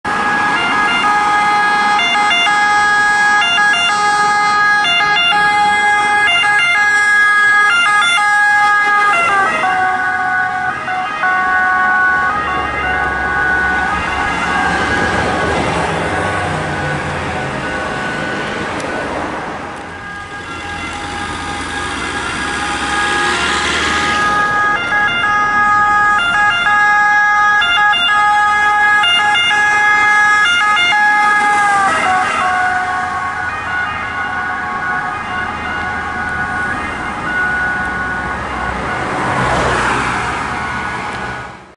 Ambulanza 13 Pubblica Assistenza Siena in Sirena
Un ambulanza della P.a. della città di Siena in arrivo al AOU (Azienda Ospedaliera universitaria) di Siena
Si ringrazia l'autista per i fai nel secondo video
Fiat ducato X290 Allestimento Cevi S.n.c.